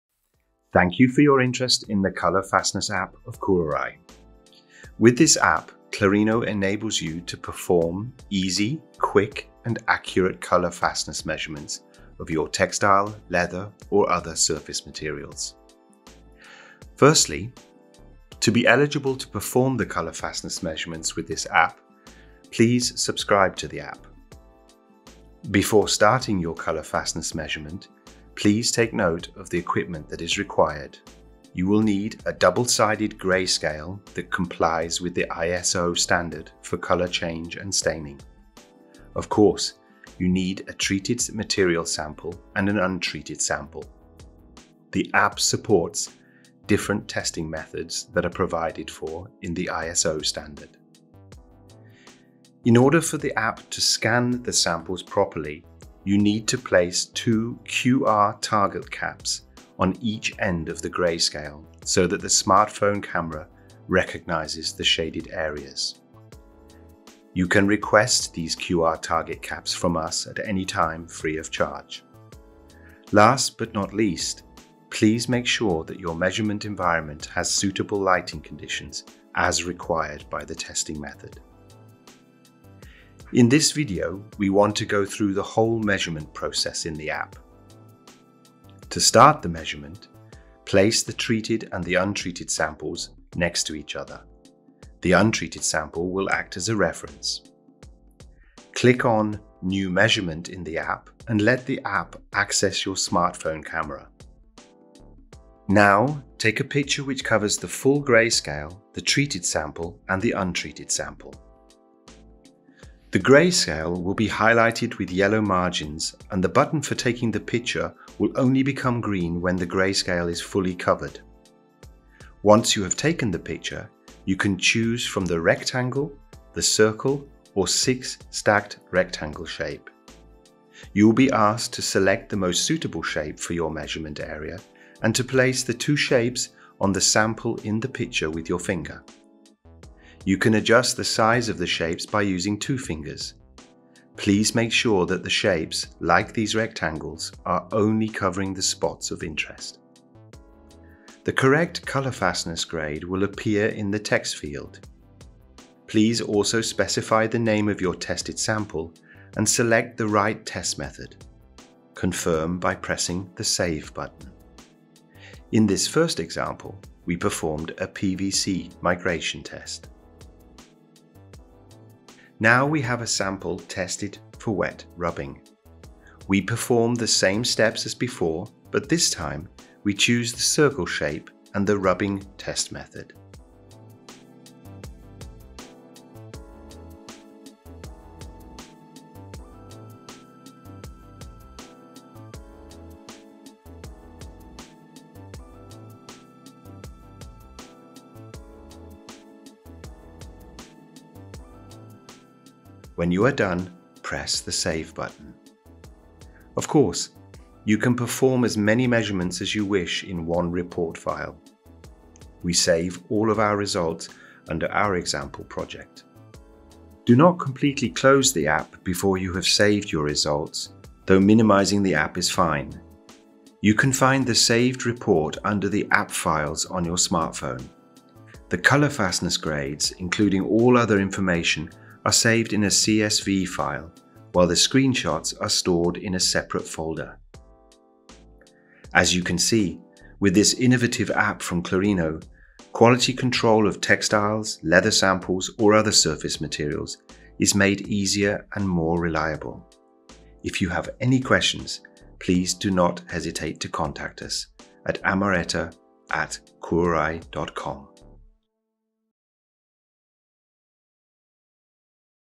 Clarino Colour Fastness App for textiles and leather
If you are active in the fashion, textile or leather industry and need to check the colour fastness of your material, this App is for you!
Colour fastness is one of the most important parameters in evaluating the quality of textiles and leather. It is the property of a dyed material to keep its colour and not to transfer its colourants to other material, e.g. while washing.The grading can be done visually or instrumentally.The visual method is prevailing, but inspectors need thorough training and eyes are prone to tiredness and subjective evaluation. There also exists digital imaging devices and spectrophotometers, which aim to compensate the drawbacks of the visual grading. The instruments are objective, but they are costly and need to be calibrated and maintained.
Therefore, Kuraray developed an App for assessing colour fastness, which is based on a process, for which a patent application was filed. It compensates the shortcomings of the previously mentioned methods:It is objective, quick, cheap, user-friendly and requires almost no training.

Moreover, there is an easy and automated documentation of the results into CSV format.
The measurement functionality can be enabled through in-app-purchase.